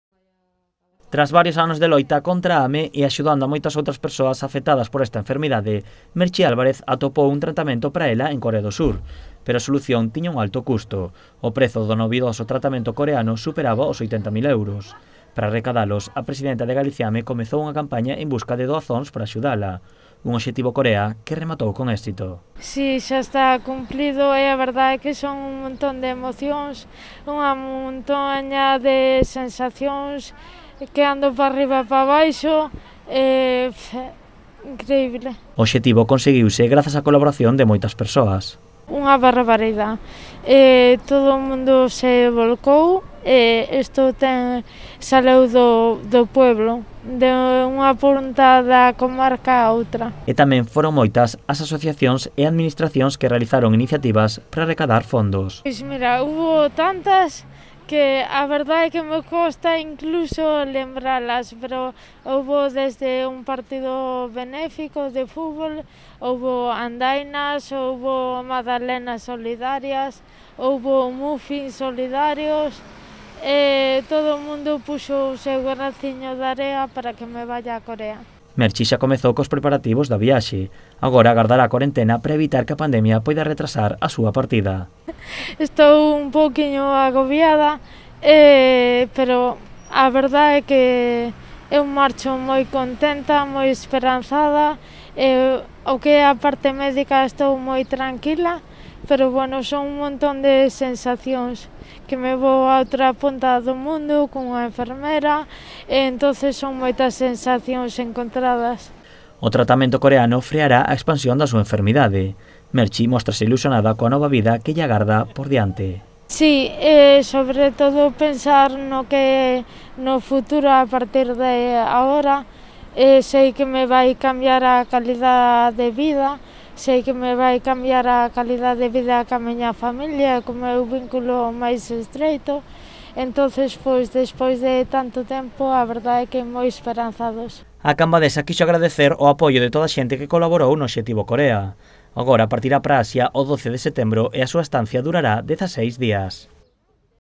Merchi Álvarez prepárase para viaxar a Corea ilusionada coa súa 'nova vida'
Merchi Álvarez acada o obxectivo Corea. A Cambadesa afectada por Ame recada os 80 mil euros necesarios para costearse o tratamento novidoso que recibirá no país asiático. A presidenta de Galiciame móstrase moi contenta e entusiasmada polo seu novo cambio de vida.